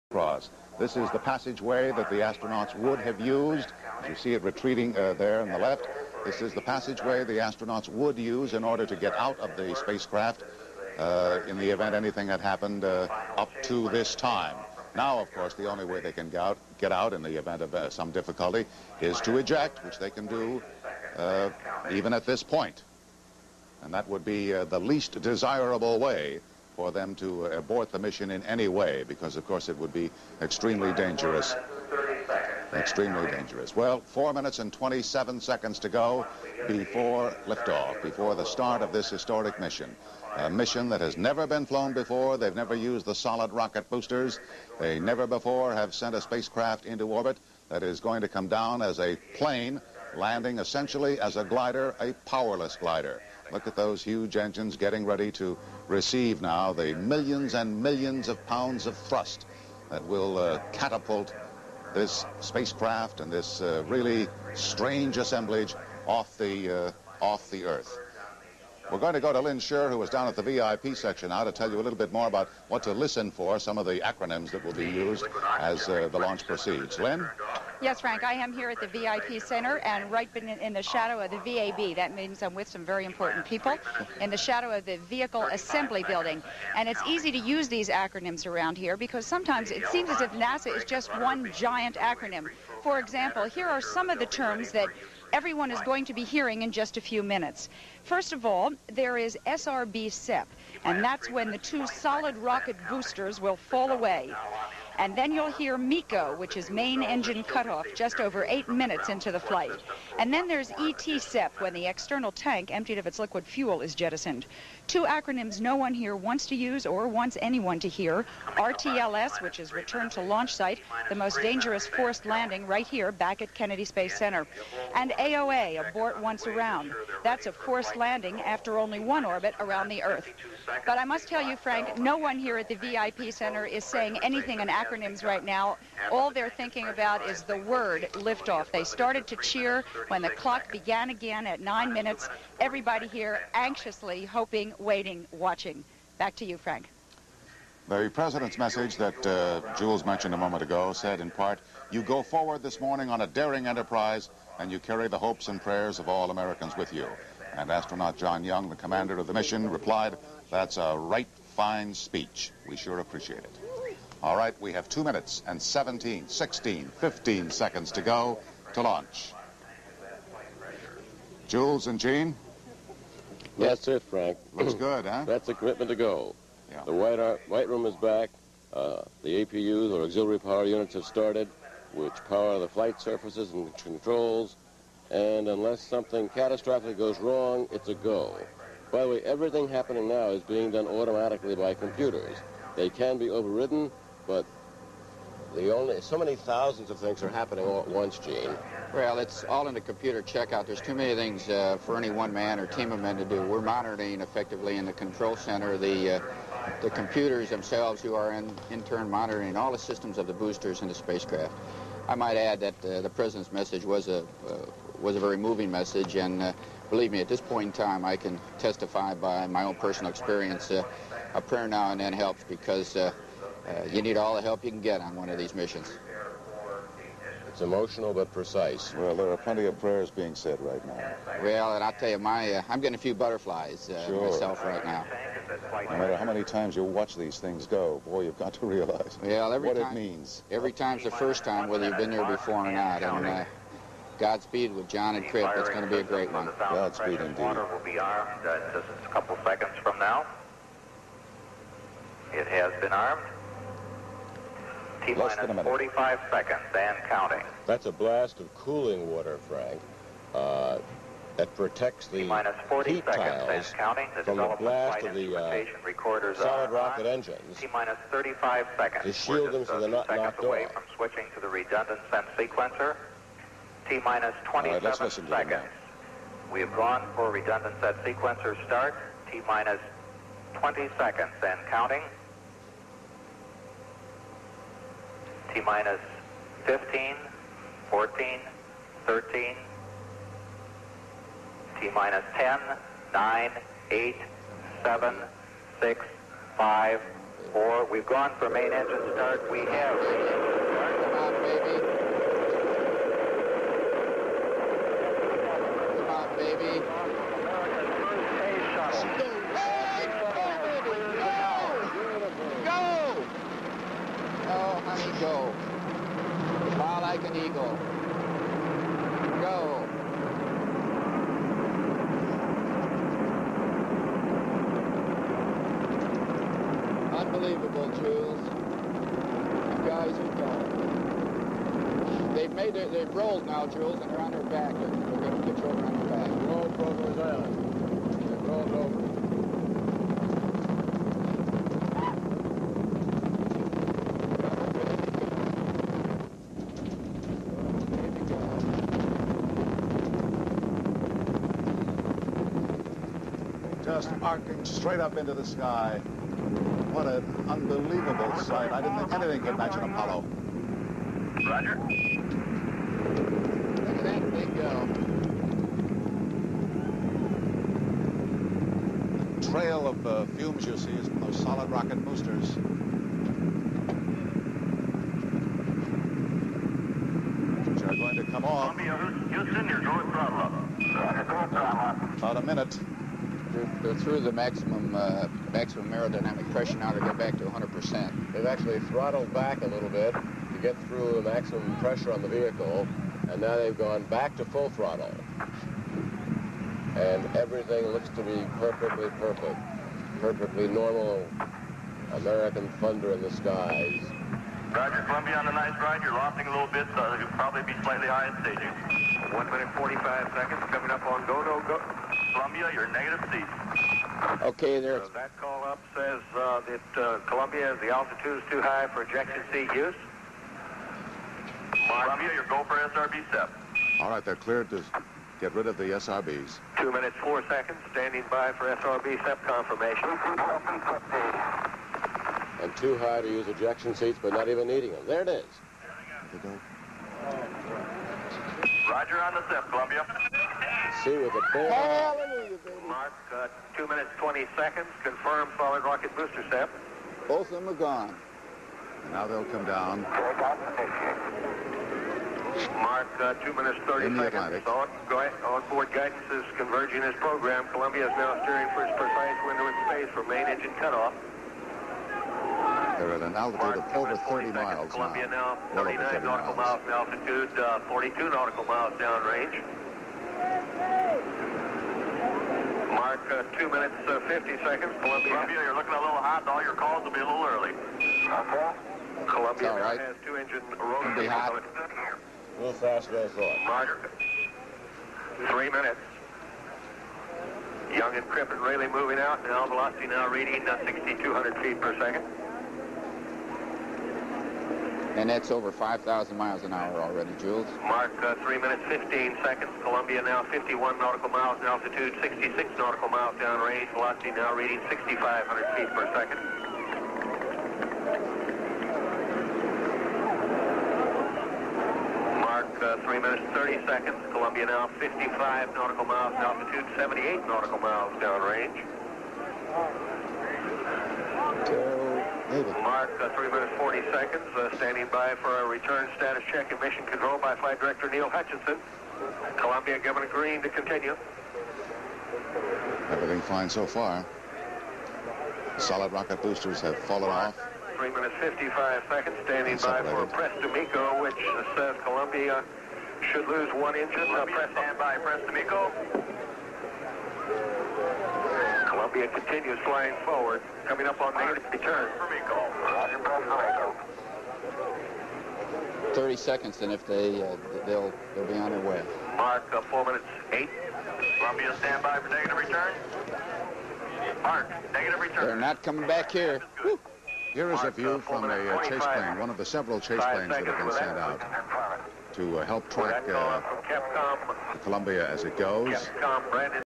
First Shuttle launch Columbia 1981
Network coverage of first shuttle launch April 12, 1981.